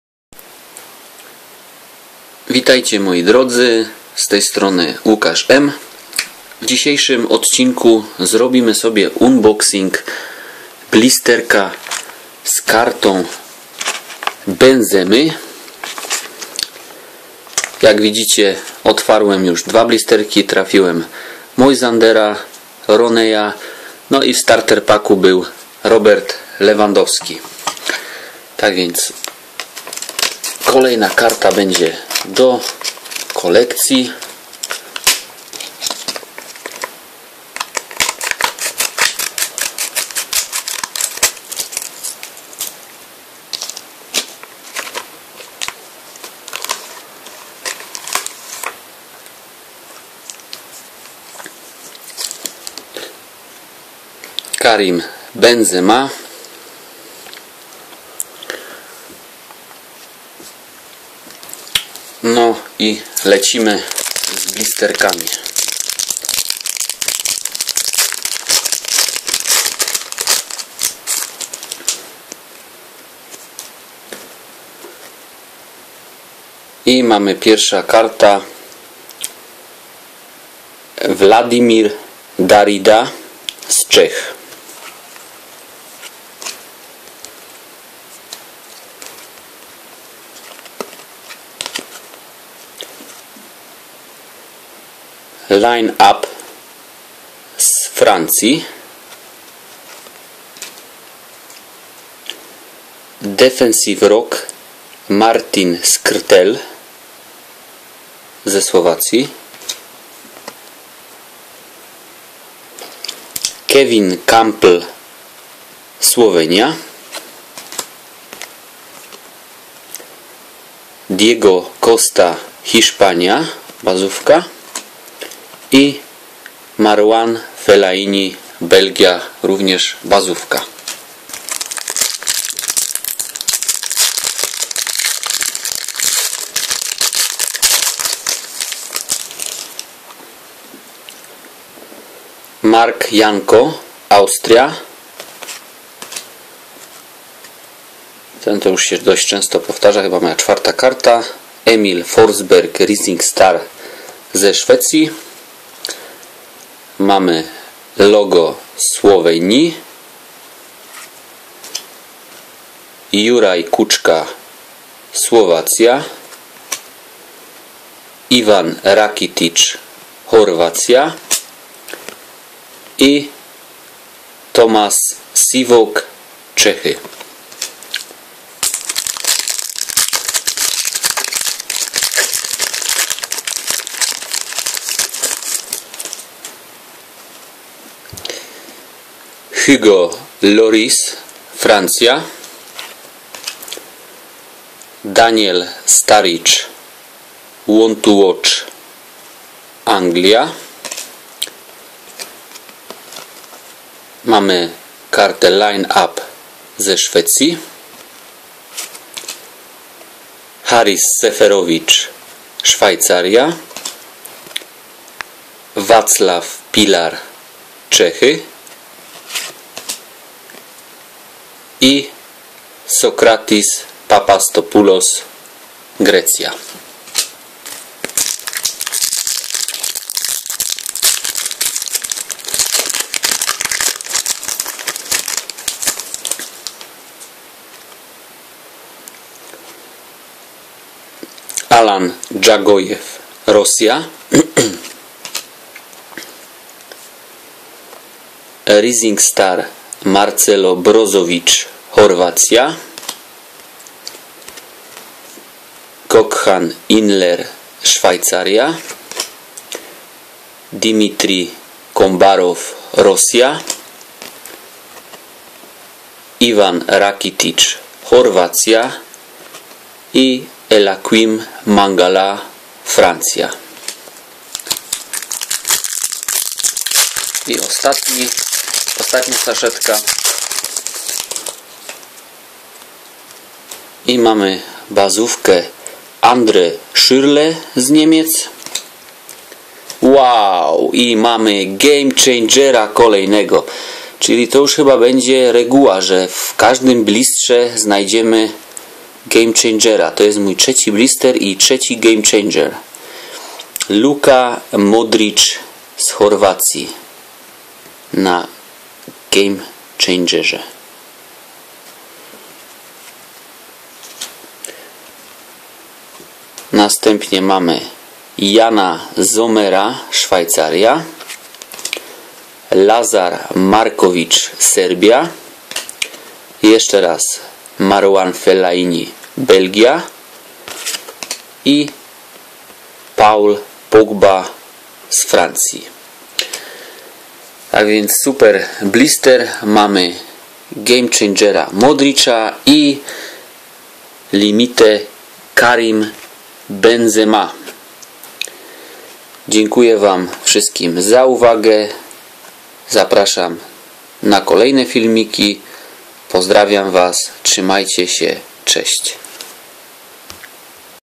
Unboxing blistra ROAD TO EURO z kartą limitowaną Karima Benzemy
Zaskoczenie...? chyba nie, powoli staje się to regułą że trafiamy w blistrze pewna karte...a jaka zobaczcie sami...